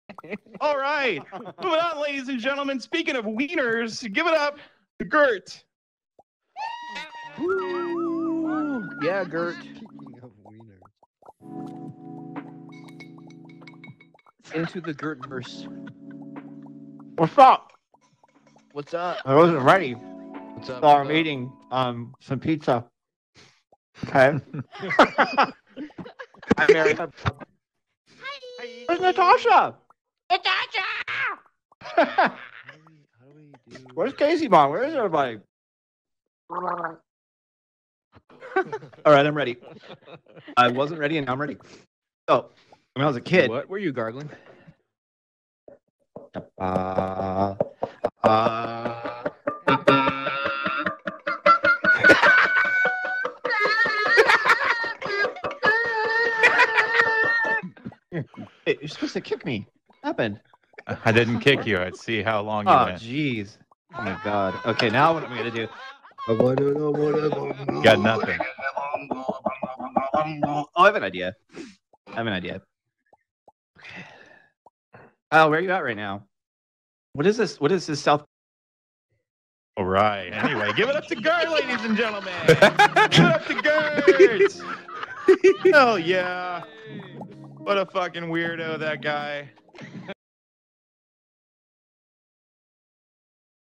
FUNNY VR COMEDY OPEN MIC HILARIOUS! AT FAILED TO RENDER COMEDY CLUB
I CAN'T STOP LAUGHING ABOUT THIS HAHA! HOSTED BY KYLE RENDER ON THIS NIGHT! FUNNY, HILARIOUS, VIRTUAL REALITY STAND-UP COMEDY AT FAILED TO RENDER COMEDY CLUB! HAHAHA